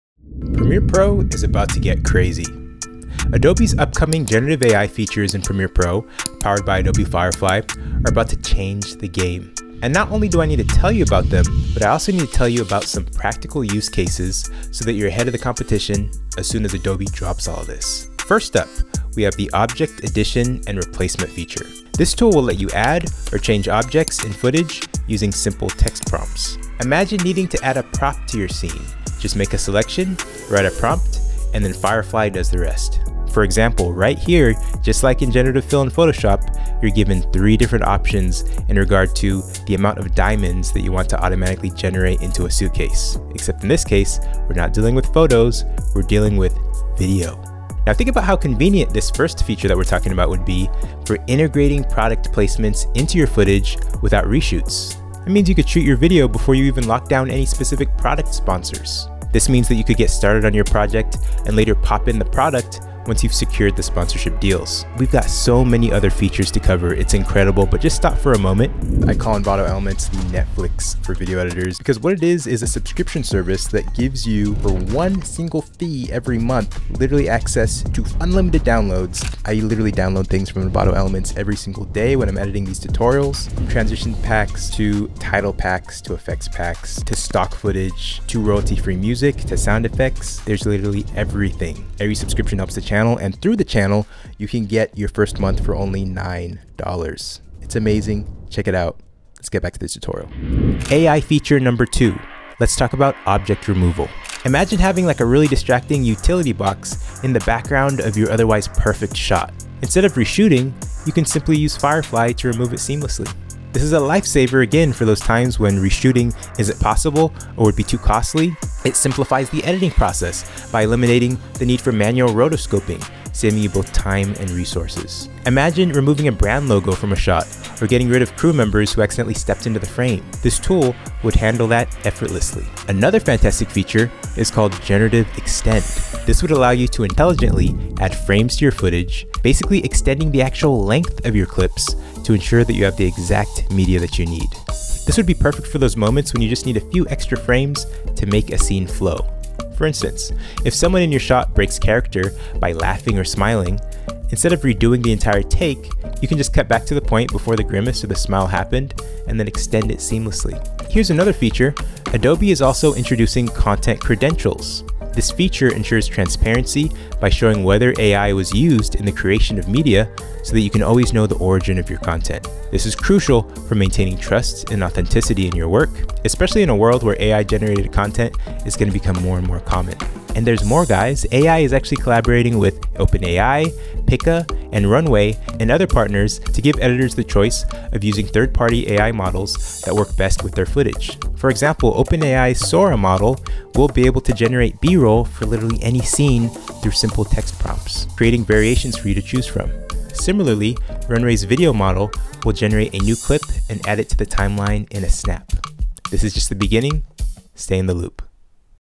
Premiere Pro Is About To Get Crazy
Premiere Pro is about to change more in the next year, than in any time in HISTORY. With new AI features set to turn Premiere Pro into more than just editing software. Are you ready?